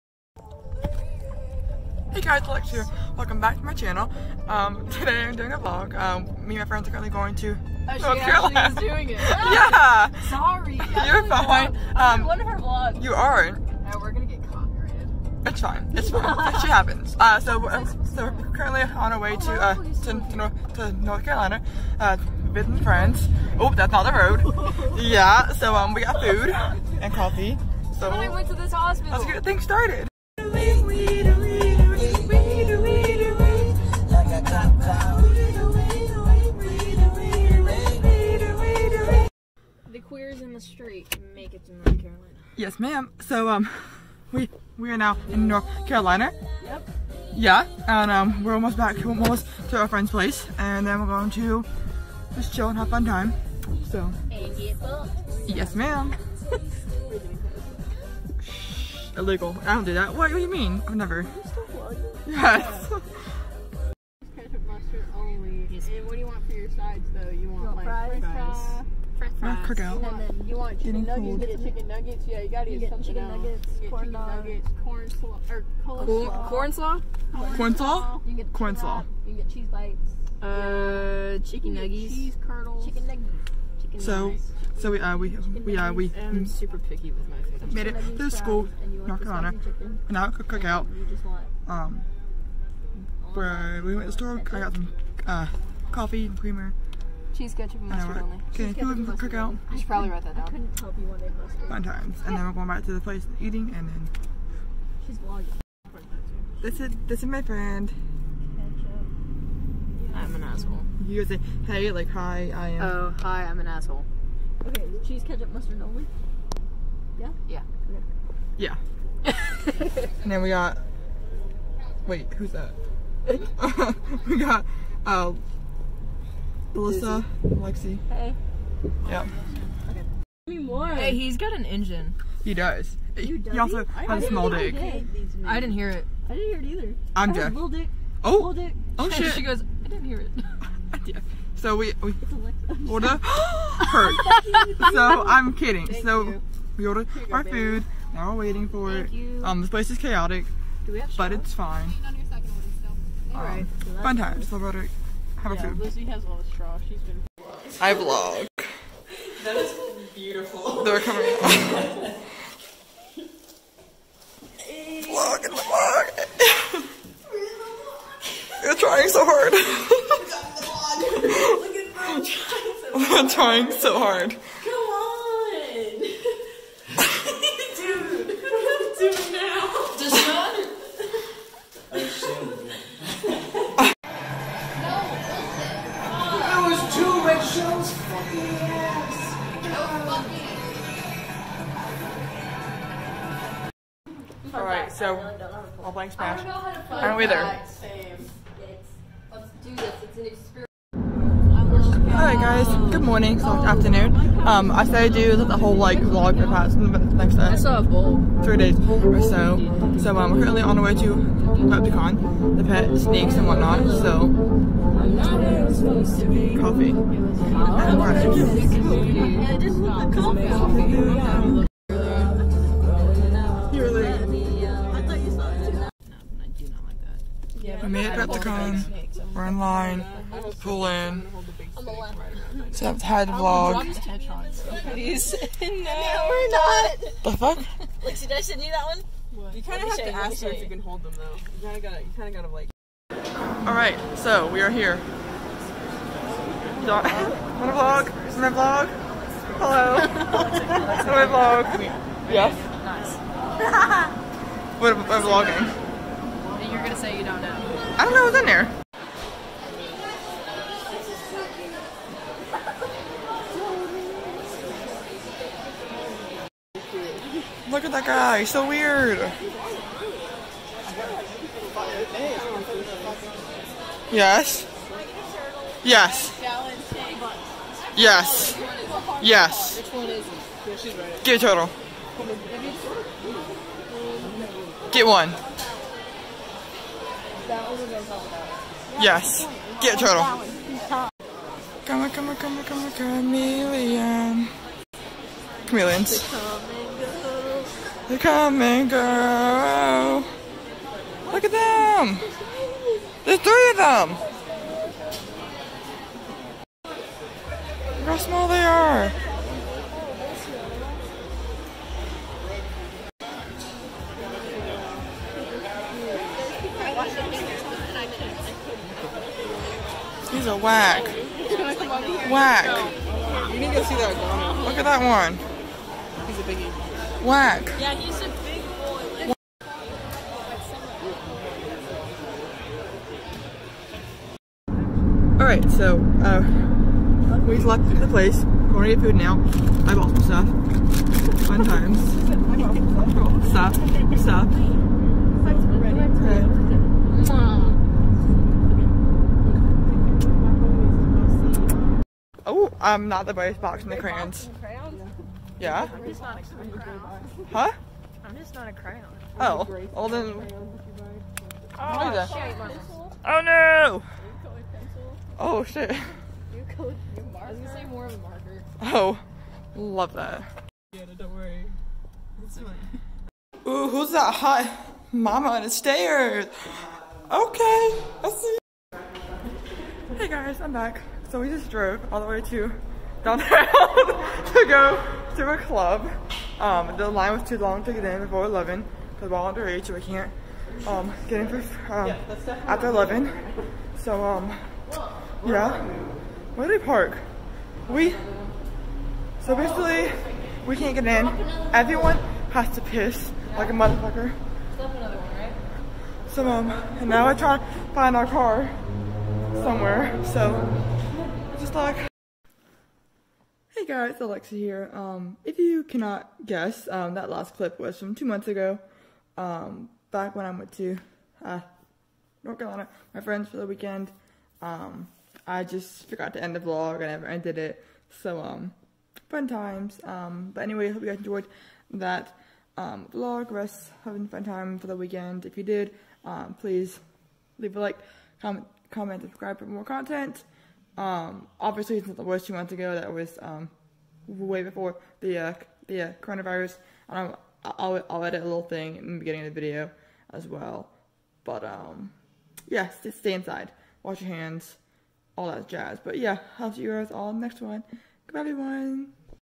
Weekend in North Carolina with my Friends (Warning: we're chaotic) | Vlog #36
Hey guys, Alexa here.

Quick disclaimer: This video was recorded months ago back when we were still in school around March before the pandemic got as large as it did and we were sent home from school. Please wear a mask when you go out now and wash your hands often.

In today's video, I have bits and pieces of a vlog of my friends and I going to North Carolina for the weekend. We had a ton of fun and it was an amazing trip. We're also chaotic as hell so enjoy that!